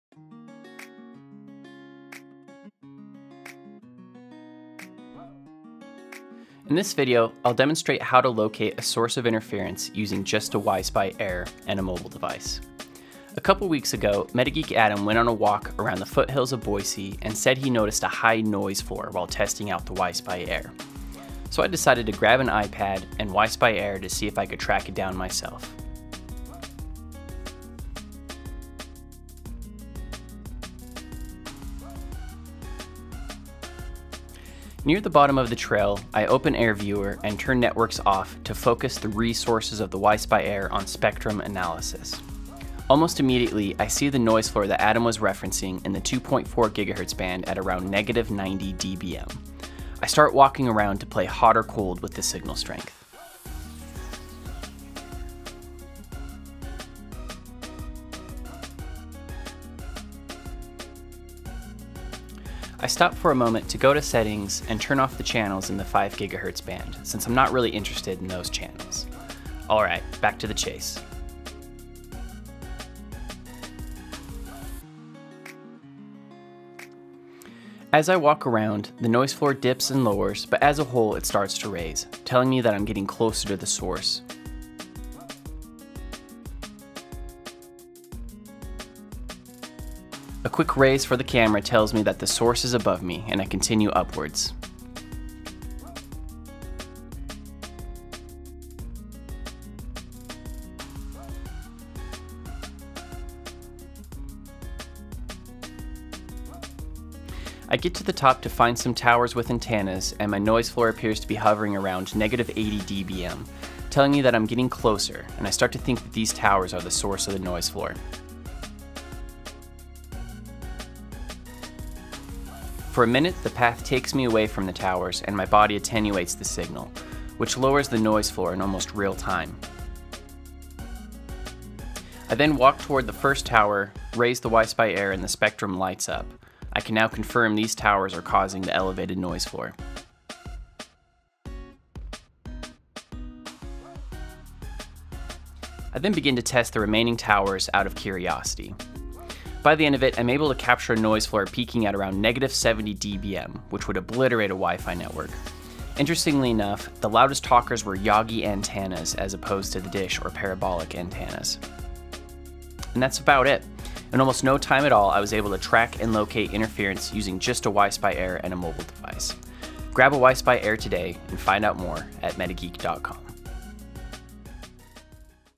Tracking interference with Wi-Spy Air!
MetaGeek Casey locates a high noise floor in the 2.4 GHz band using only a Wi-Spy Air and an iPad. MetaGeek Adam originally discovered the interference on a hike.

Wi-Spy Air is a great mobile tool to track and locate sources of RF, whether it's WiFi or not.